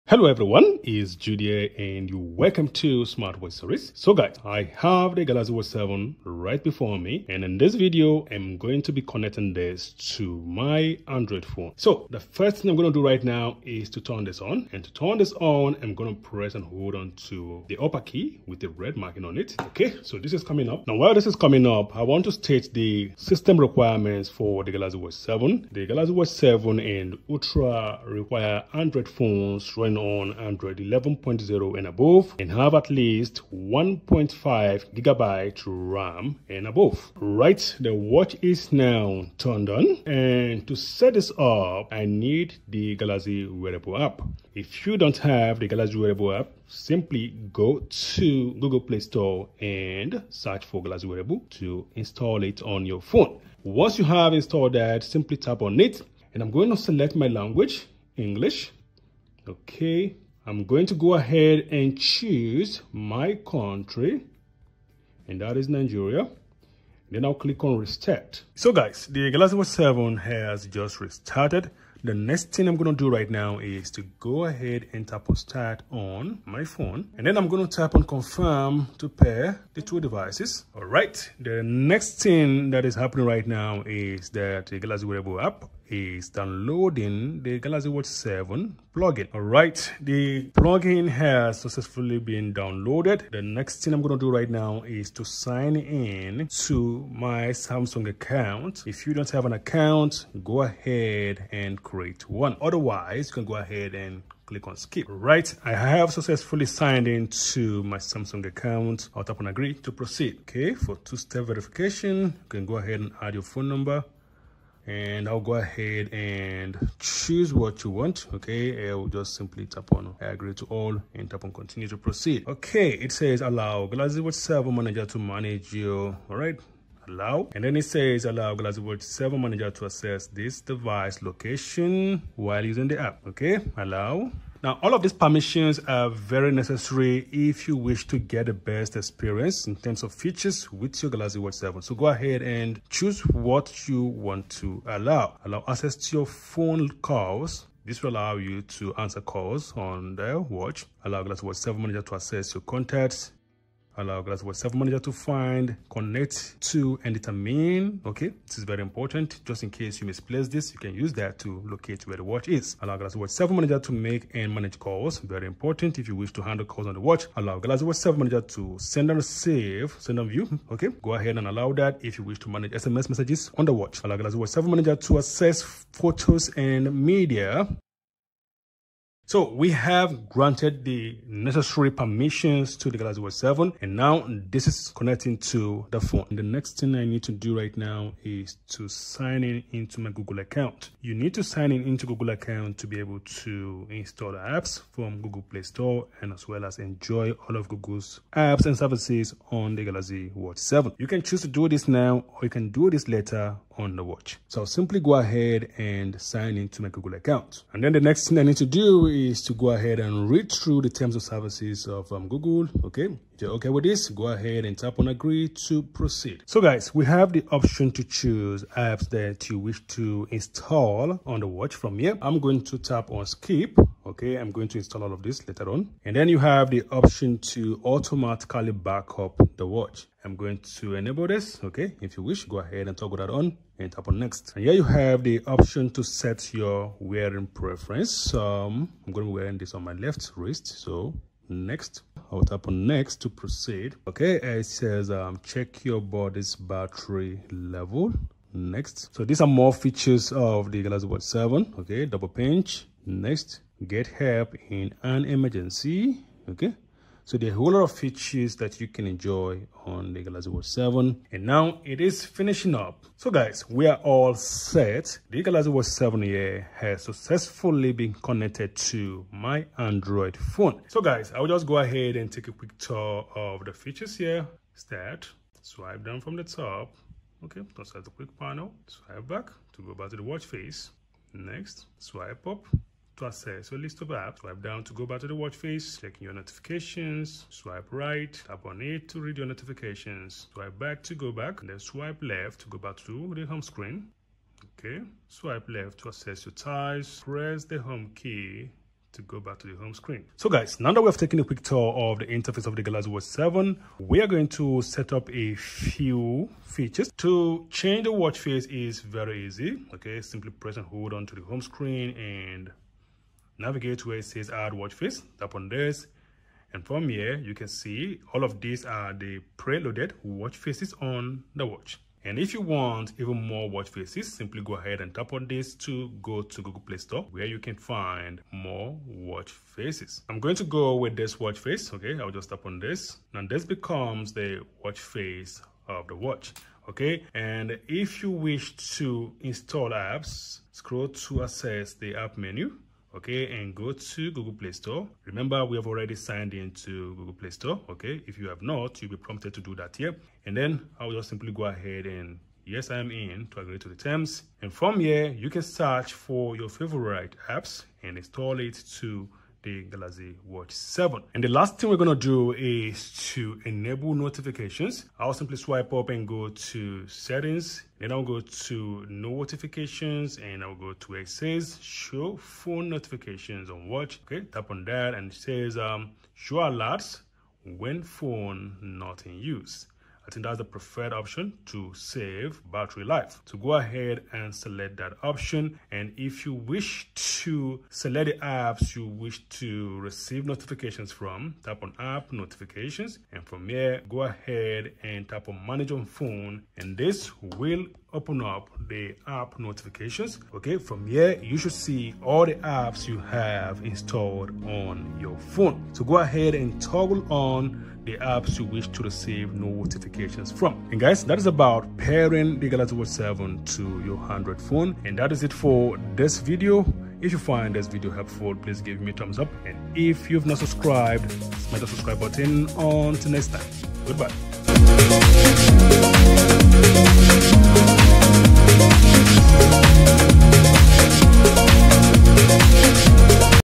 Setup and Connect Galaxy Watch 7 to Android Phone
I have just received the Galaxy Watch 7 and in this video, I walk you through how to setup and connect the Galaxy Watch 7 to your Android phone.